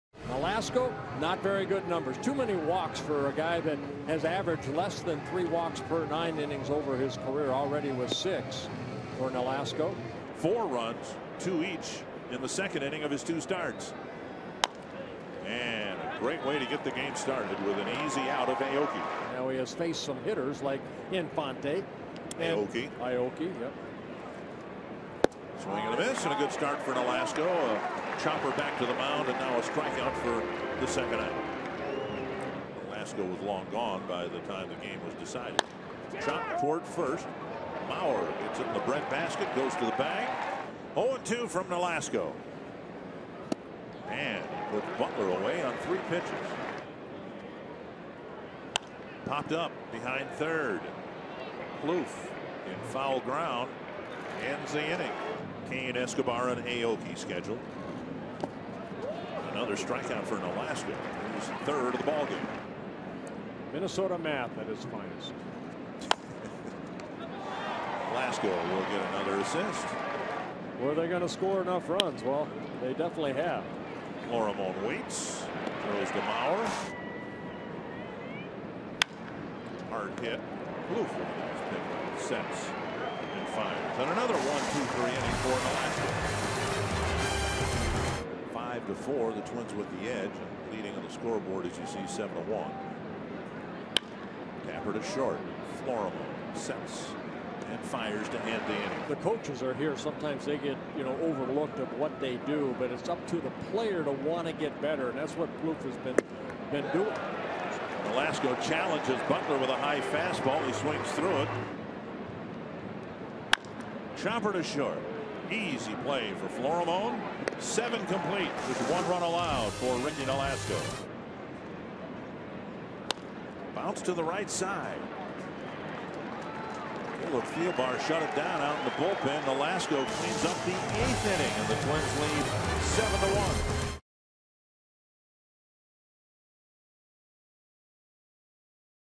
KC@MIN: Nolasco pitches eight innings of one-run ball
4/12/14: Ricky Nolasco limits the Royals to one earned run, striking out four in eight innings to earn the victory for the Twins